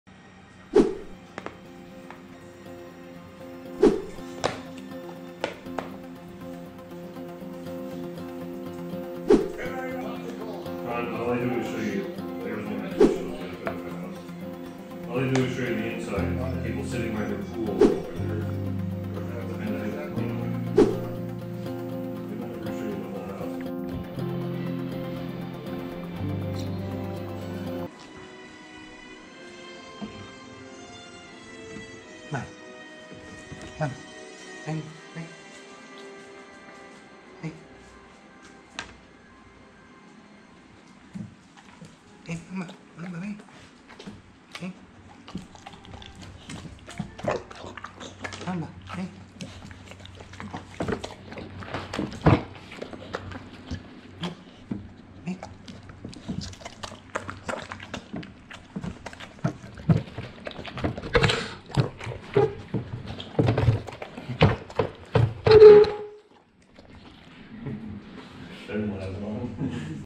Two Pitbulls dogs enjoy EATING CHICKEN gizzards ASMR
These two pitbulls dogs happily enjoy eating a delicious meal of cooked chicken gizzards.

As the pitbulls dogs continue to savor their meal, we can see their smiley and happy faces while they eat.